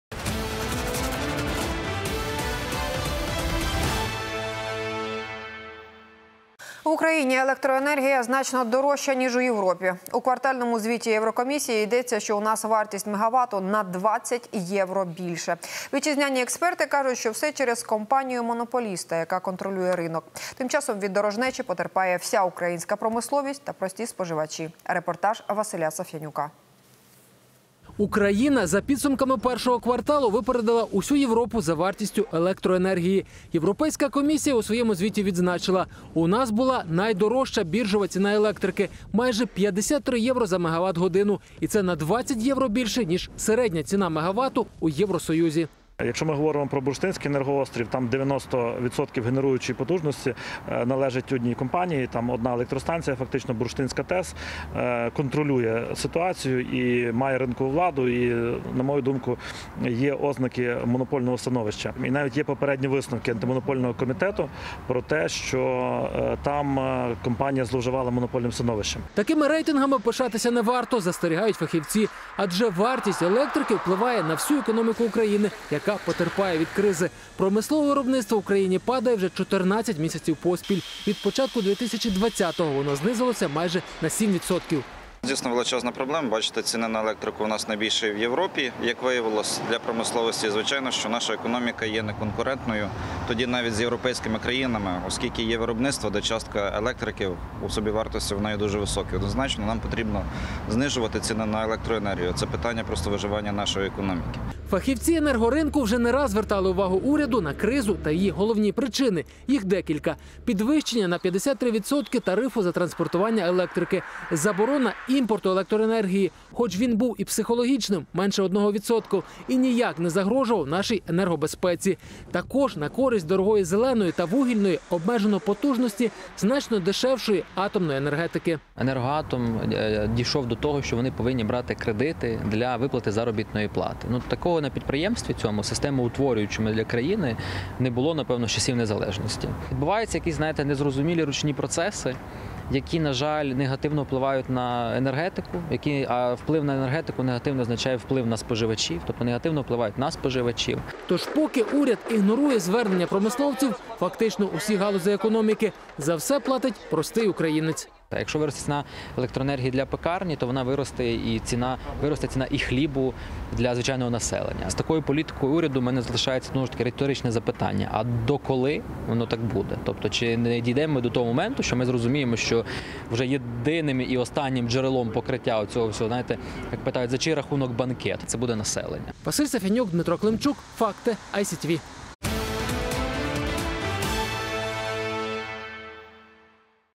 В Украине электроэнергия дороже, чем в Европе! Причина и последствия
В квартальном отчете Еврокомиссии говорится, что у нас стоимость мегаватт на 20 евро больше. Отечественные эксперты говорят, что все из-за компании-монополиста, которая контролирует рынок.

Между тем от дороговизны страдает вся украинская промышленность и простые потребители.